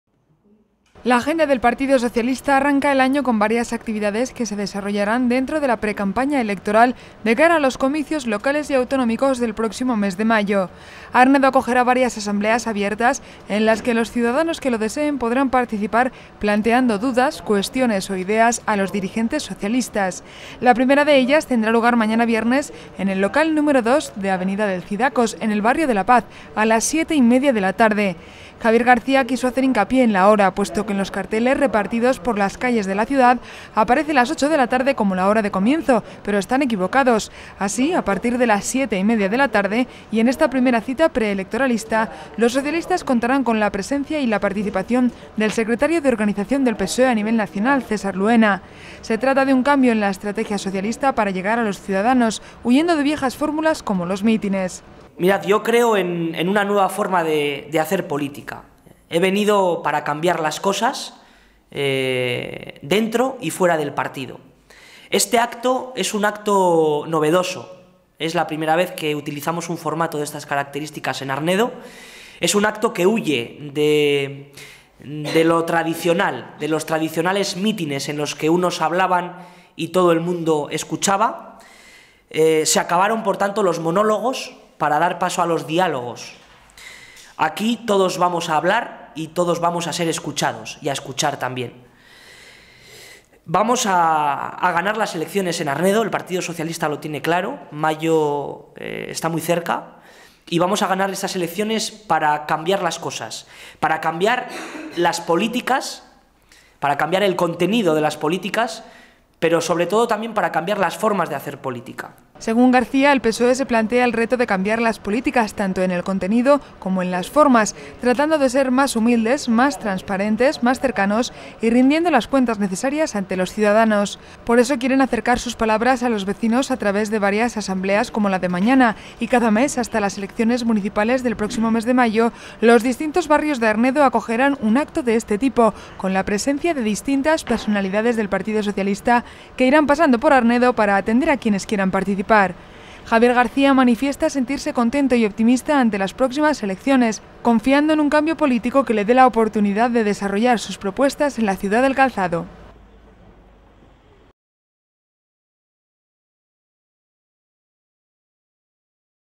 Asamblea abierta PSOE
El Partido Socialista comienza el año con fuerza, y enmarcada en la agenda de precampaña electoral, mañana en Arnedo se celebrará una asamblea abierta con el Secretario de Organización del PSOE, César Luena, y todos aquellos ciudadanos que quieran participar. Un acto que se desarrollará en el Barrio de la Paz, concretamente en el local de Avenida del Cidacos, y con el objetivo de trasladar a los ciudadanos el sentir del Partido Socialista, que trata de huir de viejas fórmulas, como los mítines.